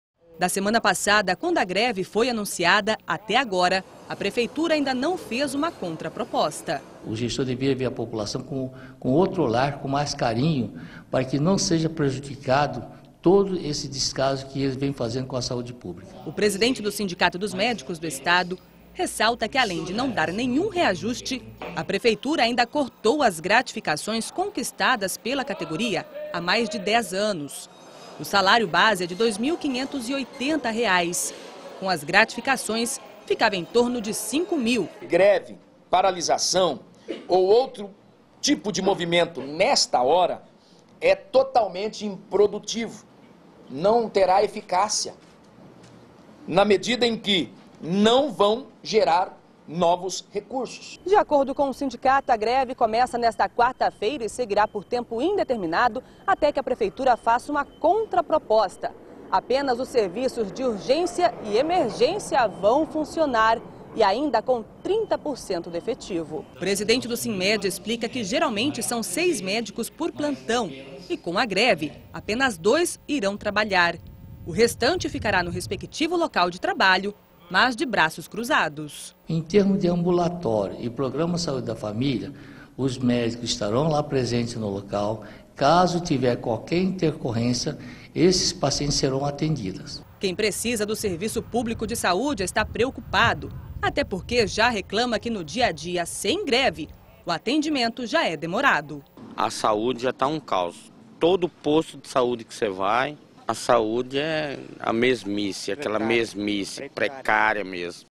Sem gratificações, médicos de postos de saúde prometem greve
A partir de amanhã, a população de Campo Grande pode enfrentar dificuldades ainda maiores que as rotineiras no atendimento nos postos de saúde. Isso por que os médicos prometem  entrar em greve. Eles reivindicam  a reposição da inflação e o retorno das gratificações.